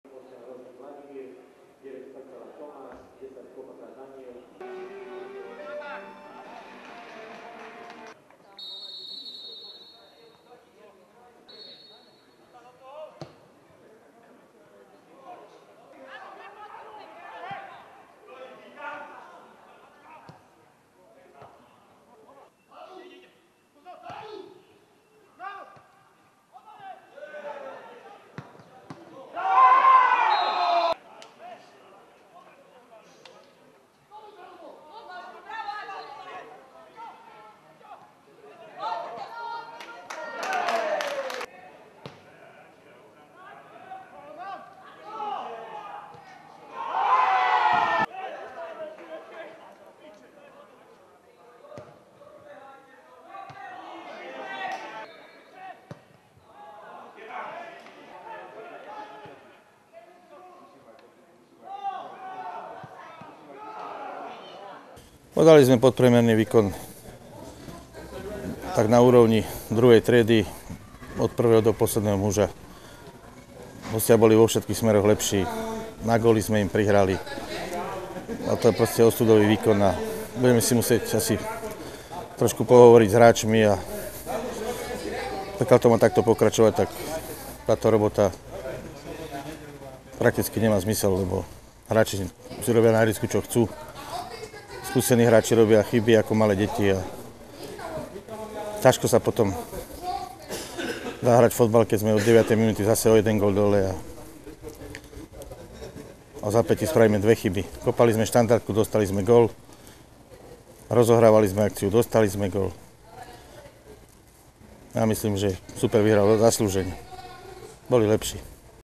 futbal Ludanice Galanta 2.10.2010
Futbal  III. liga -10. kolo
OFK Metacolor Ludanice - FC Slovan Galanta 0:2 (0:2)
Góly: Méry, Čerňanský